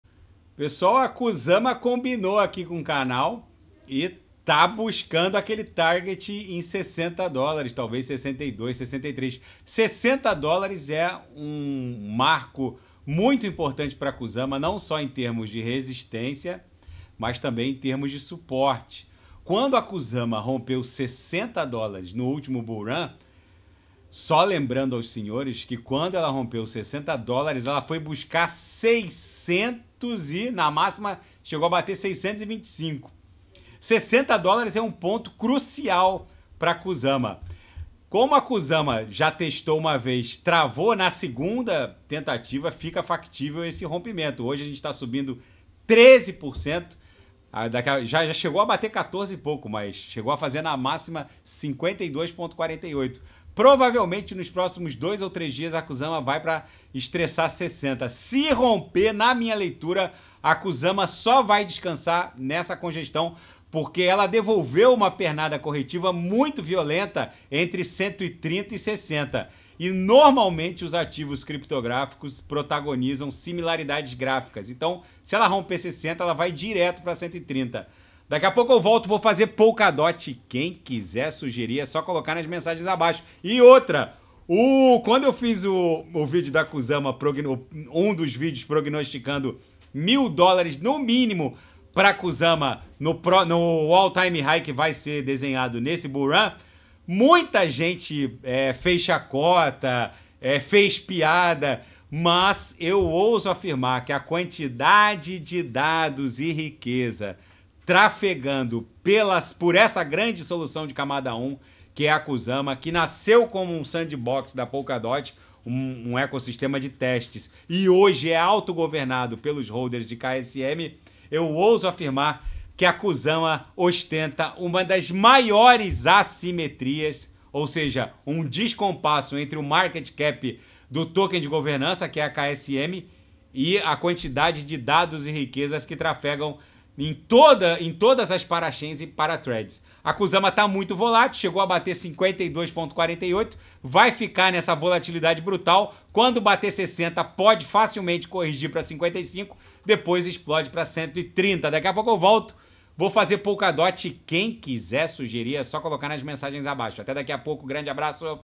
KSM - Kusama! Análise de hoje! 19/02/2024!!!
TWPvSrVQbDq4wyYiWTg2ubdqCfFhPkdiRw

Por favor, peguem essas informações e façam suas próprias pesquisas.